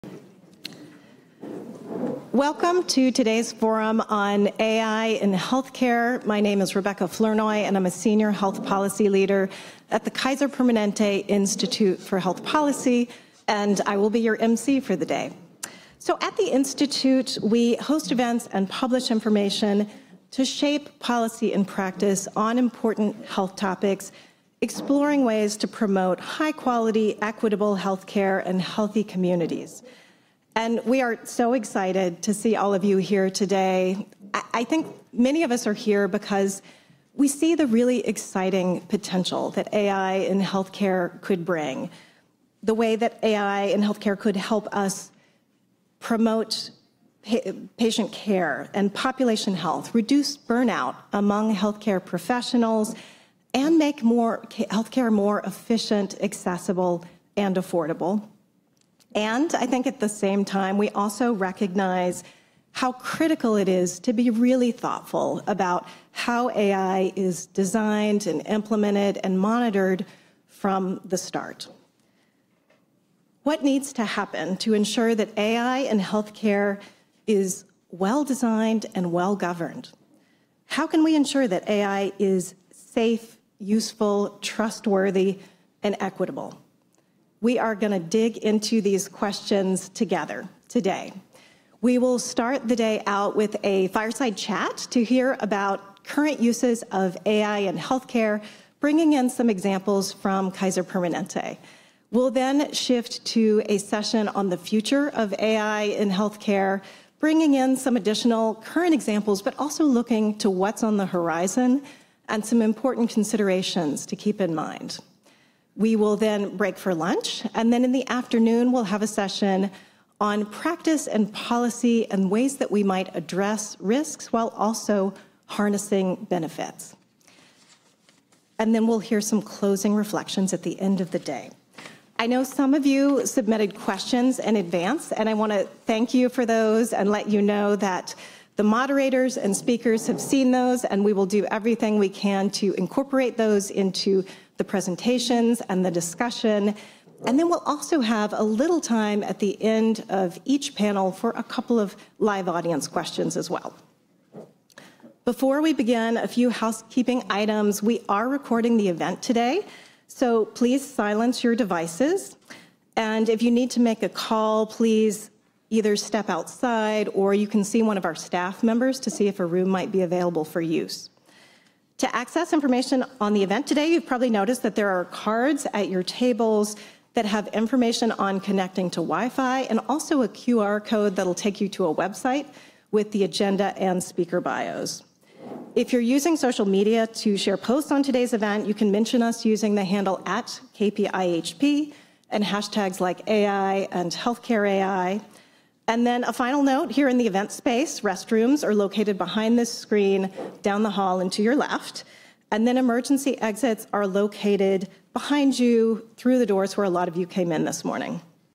AI Transforming Health Care: Trust, Equity, and Policy - Welcome | Kaiser Permanente
Institute for Health Policy Forum:
AI Transforming Health Care: Trust, Equity, and Policy

Speaker:
Rebecca Flournoy, MPH
Senior Health Policy Leader
Kaiser Permanente Institute for Health Policy